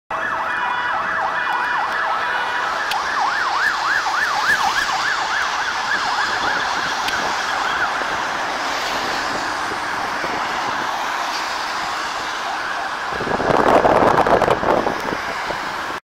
Alarmowo nowa karetka Renault Master IV P8 Pogotowie Ratunkowe w Warszawie
Nowy ambulans PR Warszawa alarmowo do interwencji.